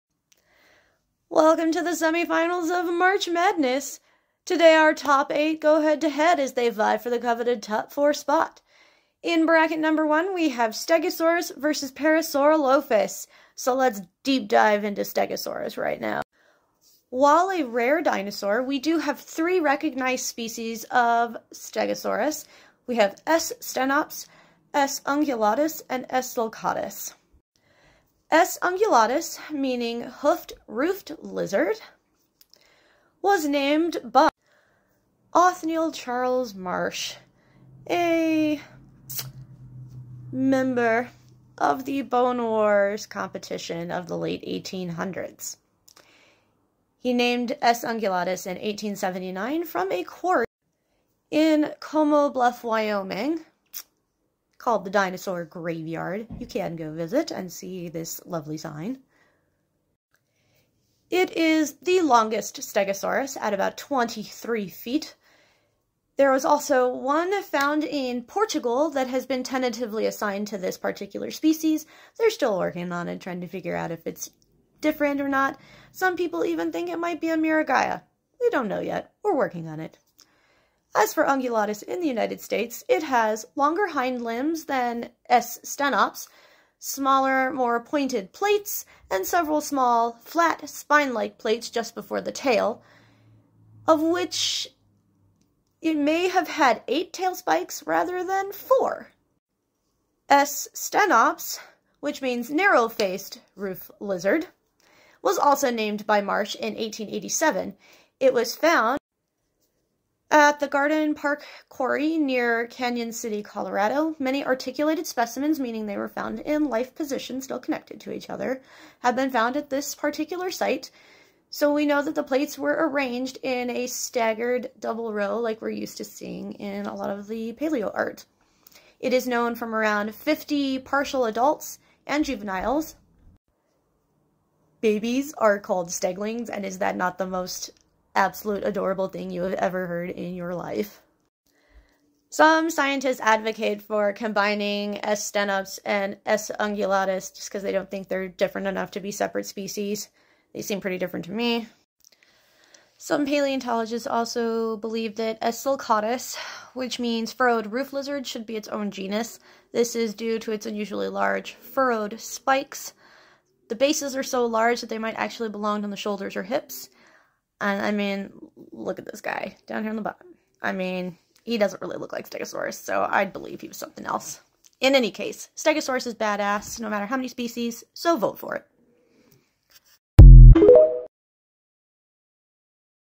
March Madness Semi Finals: Stegosaurus
March Madness Semi Finals Have Begun! Bracket 1, competitor 1 is Stegosaurus. If you want Stegosaurus to move on to the Top Four, like this video! There will be 8 total videos, one for each competitor, so keep your eyes peeled!